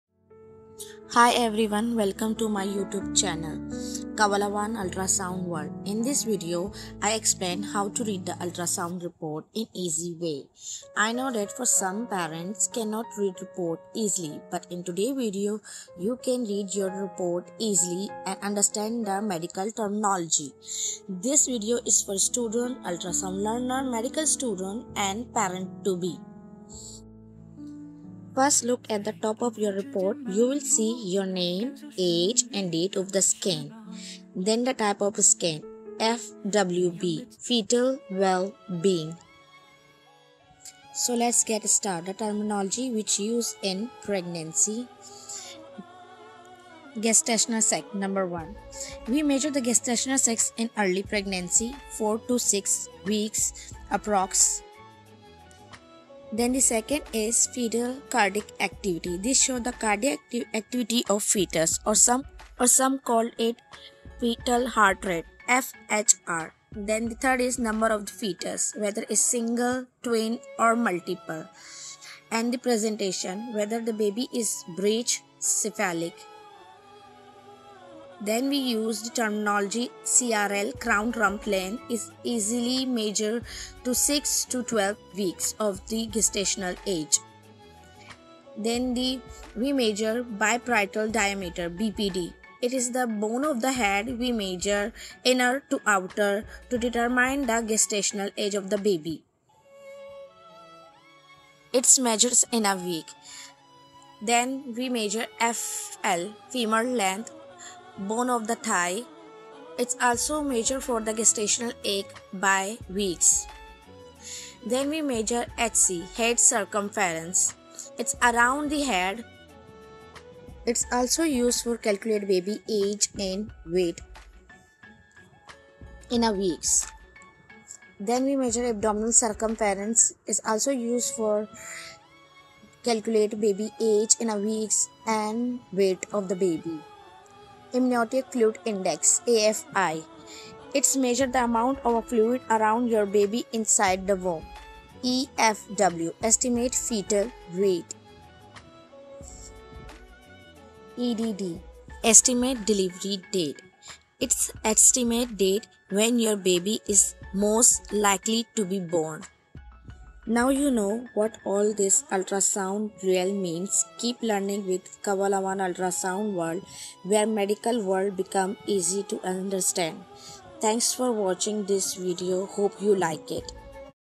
How to Read (FWB) ultrasound report in easy way| Explained in step by step
How to Read FWB (Fetal Wellbeing) Report Easily | Pregnancy Ultrasound Explained

Confused by your FWB (Fetal Wellbeing) ultrasound report? This video makes it simple! Learn how to read every part of the FWB report in easy language – from baby measurements like BPD,GS,CRL HC, AC, FL,EDD,EFW to amniotic fluid, placenta, and overall wellbeing. Perfect guide for expecting parents!

🔹 Understand FWB report step by step
🔹 Know what all abbreviations and numbers mean
🔹 Make sense of your baby’s growth and health